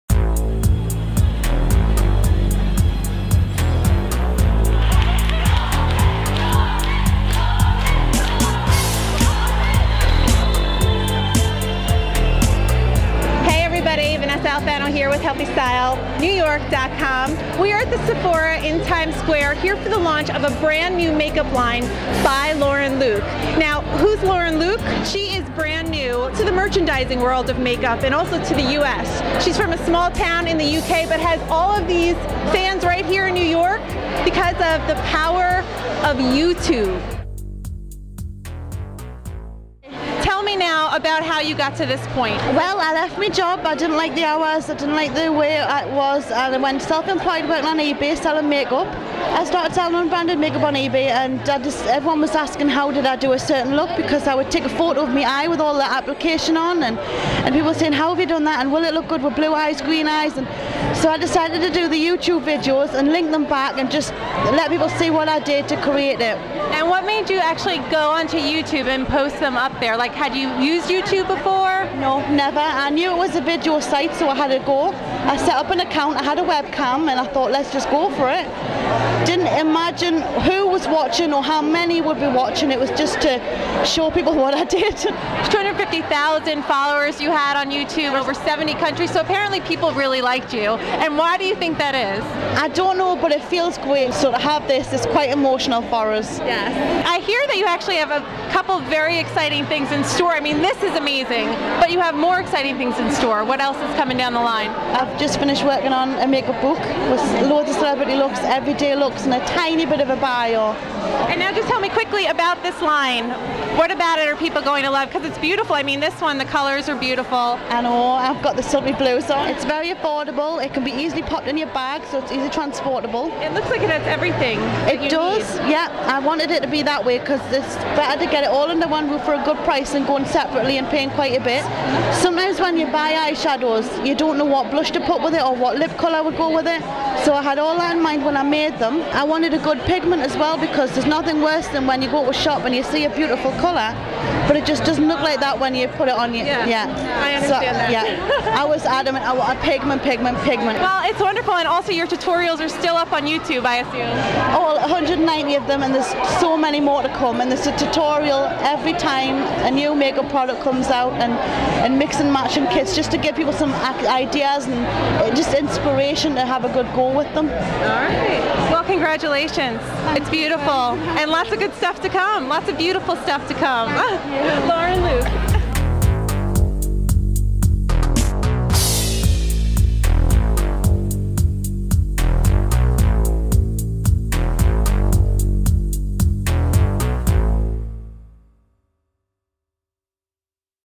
Lauren Luke & The Power of Youtube (HealthyStyleNY.com)
Vanessa Alfano of HealthyStyleNY.com interviews Lauren Luke at the launch of her makeup line ByLaurenLuke at Sephora in times Square.